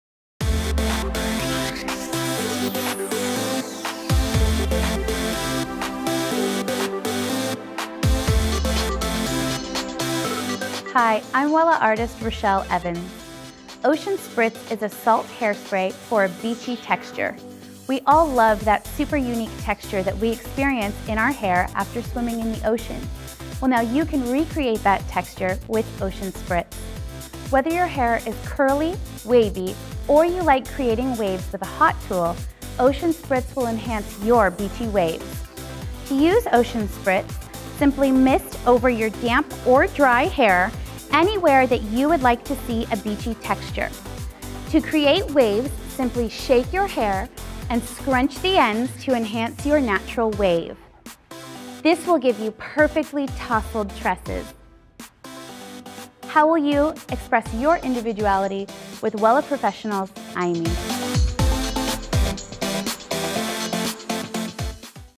Wella Professionals EIMI Ocean Spritz Sea Salt Spray
Wella EIMI Ocean Spritz is a salt spray for an undone beachy texture style with a light hold.